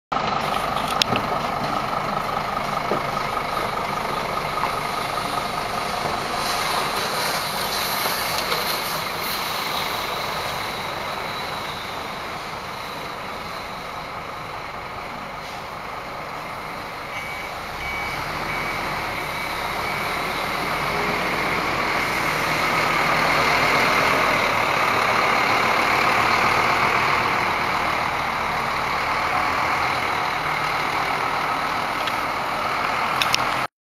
Avgång lasten den 31 december 2014
Vår last går iväg med över 800 kg mat m.m..

Tusen tack till Leif, Liselott & Octavian som gjorde detta möjligt.